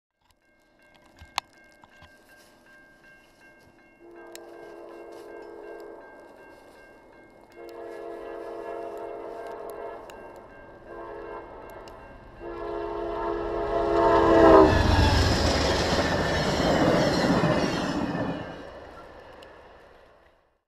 Via 41 at dawn
As the sun barely begins to rise, Via 41 makes its way west at 90 mph as it approaches Mallorytown. The RailfanBrothers left their home town of Ottawa at 5:30 am to begin what would end up being a unique day of rail fanning. As mother nature had a nasty surprise waiting for us. December 20th 2013